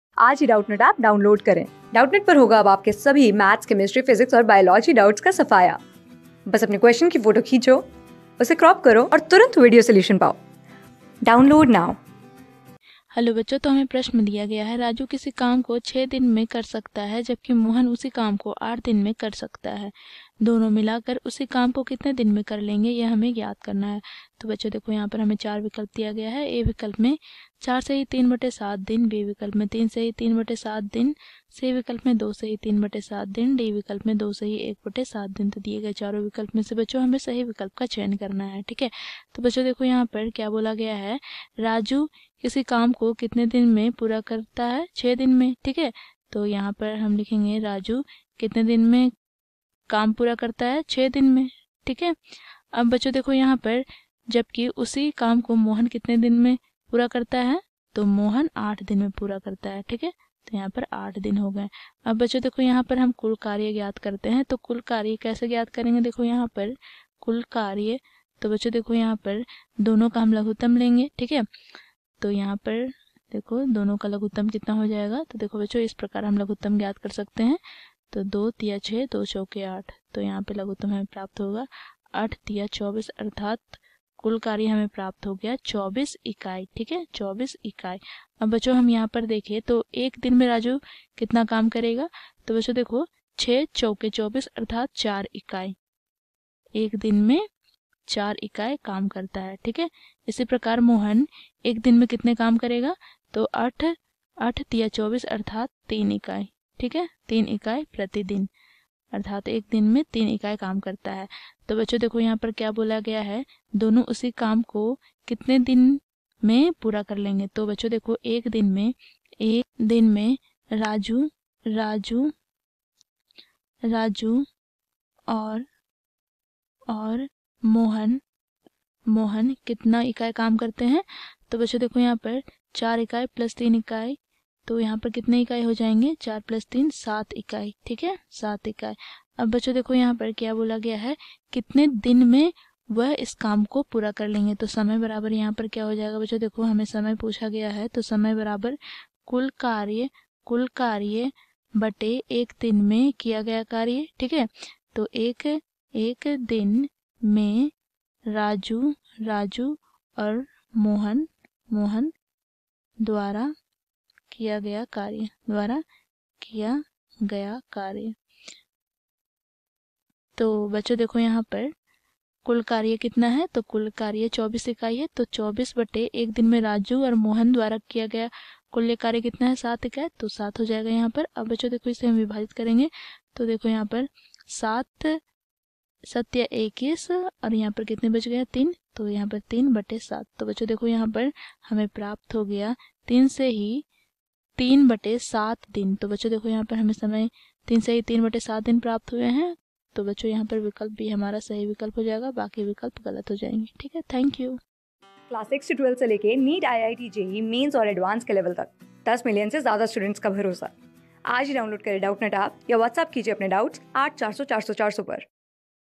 राजू किसी काम को 6 दिन में कर सकता है जबकि मोहन उसी काम को 8 दिन में कर सकता है। दोनों मिलकर उसी...
राजू किसी काम को 6 दिन में कर सकता है जबकि मोहन उसी काम को 8 दिन में कर सकता है। दोनों मिलकर उसी काम को कितने दिन में कर लेंगे?
Class: 14
Subject: MATHS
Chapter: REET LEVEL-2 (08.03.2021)
Board:CTET

👉 Have Any Query? Ask Us.